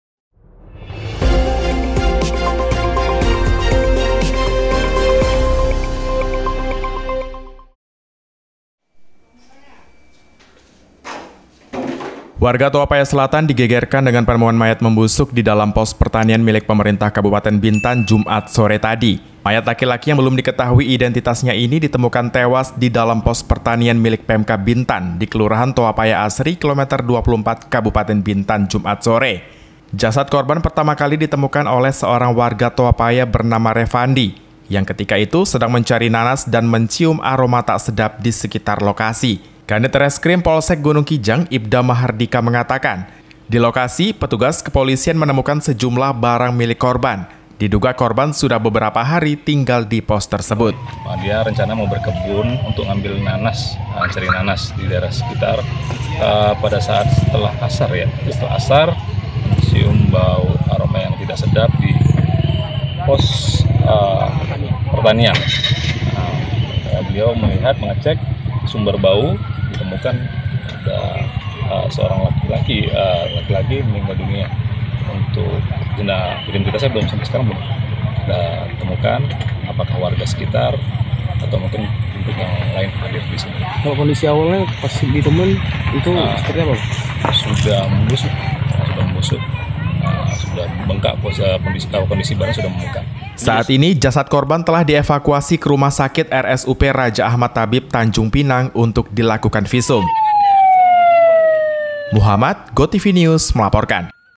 Geger! Warga Toapaya Temukan Mayat Membusuk Dalam Pos Pertanian
GOTVNEWS, Bintan - Warga Toapaya Selatan digegerkan dengan penemuan mayat membusuk di dalam pos pertanian milik Pemerintah Kabupaten Bintan, Jumat sore tadi.(Mhd)